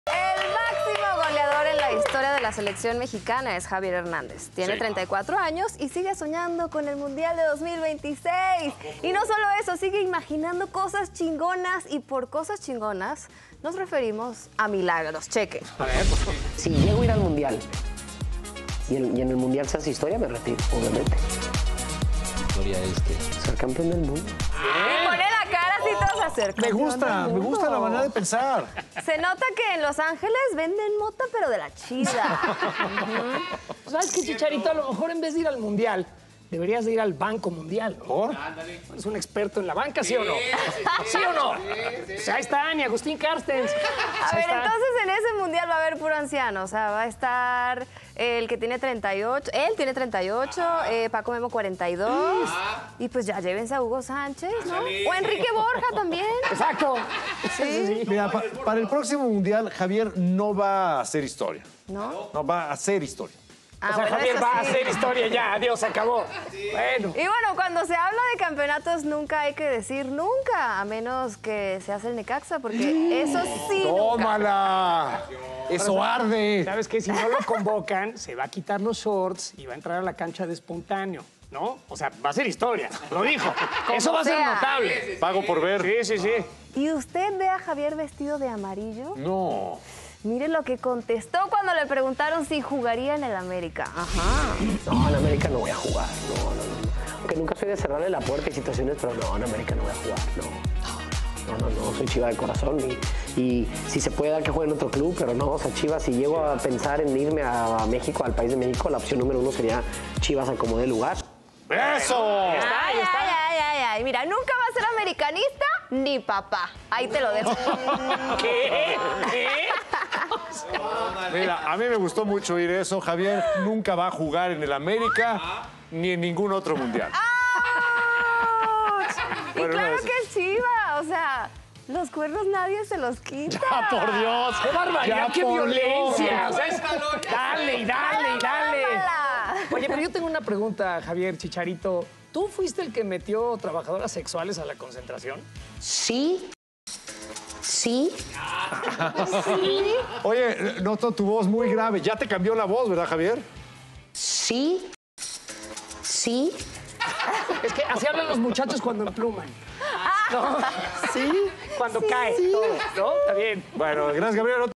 "Chicharito" le hace el feo al América | Qué Importa
El Chicharito le hace el feo al América y sueña con jugar otro Mundial y ser campeón con México.